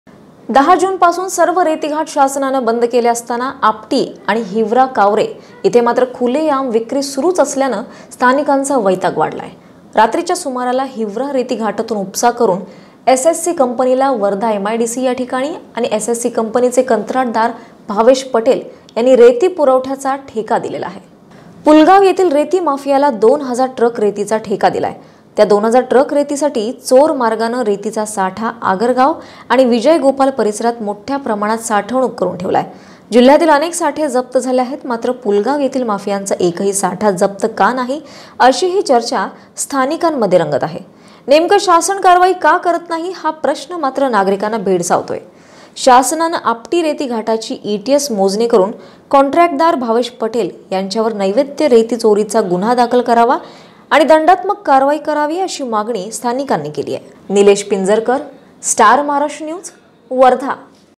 वर्धा-रेती माफियांना रेती भरलेला ट्रक पेटवला
. वर्धा जिल्ह्यात चोर मार्गाने  रेतीचा साठा आगरगाव आणि  विजय गोपाल परिसरात मोठ्या प्रमाणात साठवणूक करून ठेवला  आहे जिल्ह्यातील अनेक साठे जप्त झाले आहेत  मात्र पुलगाव येथील माफियांचा एकही साठा जप्त का नाही अशीही चर्चा स्थानिकांनमध्ये  रंगत आहे.